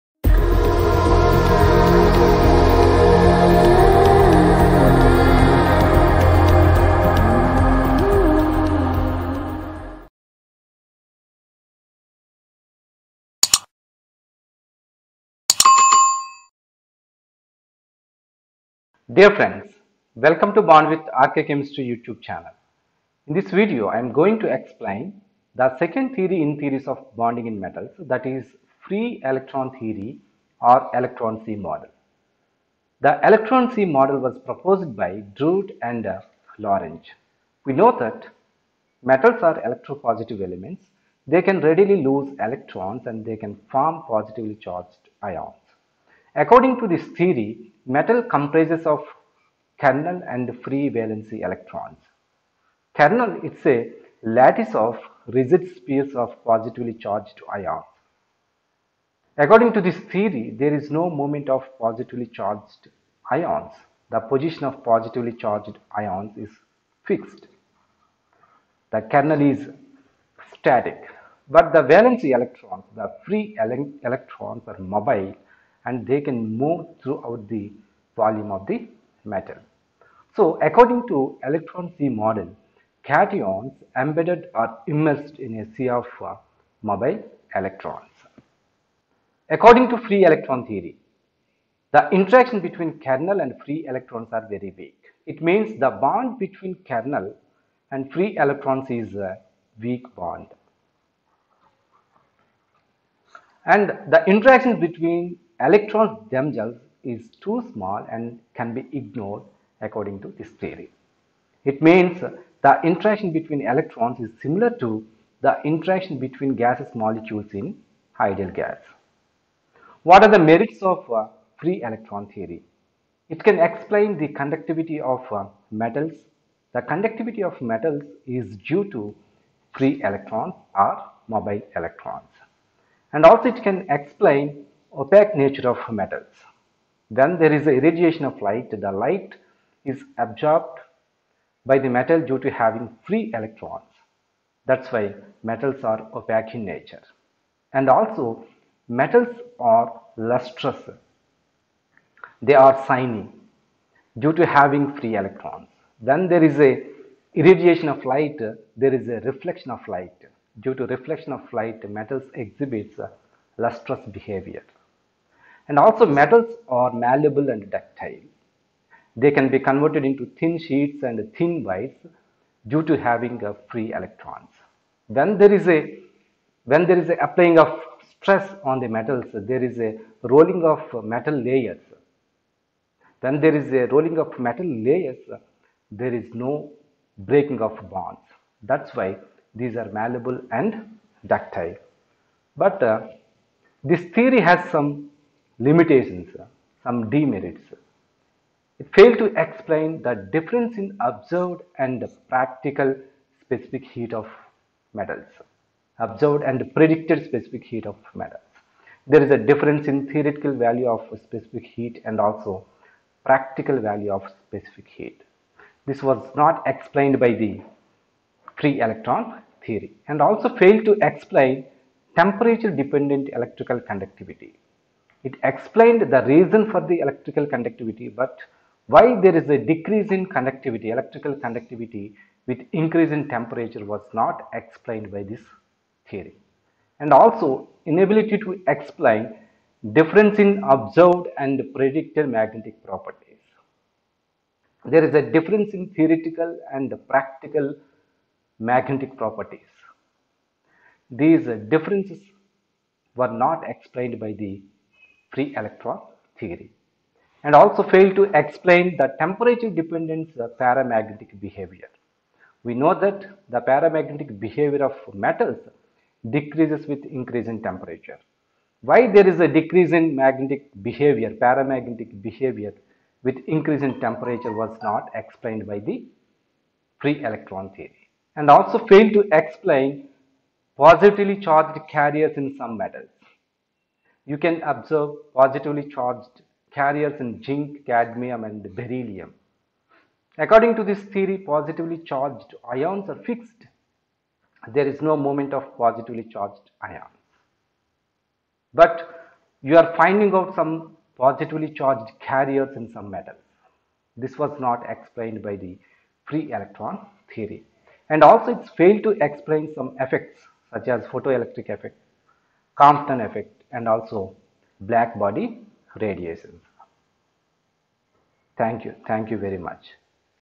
Free Electron Theory || Electron Sea Model || Theories of Bonding in Metals || Bond with RK
Free Electron Theory
Proposed by Drude & Lorentz
Metal comprises of kernel & free valency      electrons
Kernel : Lattice of rigid spheres (positive ions)
Free valence electrons: Move all around        the volume of metal
Cations immersed in a sea of mobile electrons
The interaction between kernel and free electrons are weak
The interactions between electrons themselves is too small and can be ignored
Merits:
Explained the following properties of metals
Conductivity is due to free electrons
Opaque nature of metals
Lustrous behavior of metals
Malleability & ductility
Limitations:
Inability to explain the difference in observed and predicted specific heat of metals
Fail to explain ‘T’ dependent electrical conductivity
Inability to explain the difference in observed and predicted magnetic properties.
Fail to explain ‘T’ dependance paramagnetic behavior
Fail to explain positively charged carriers in some metals ( Zn, Cd, Be)
Fail to explain Photoelectric Effect, Compton Effect & Black body radiation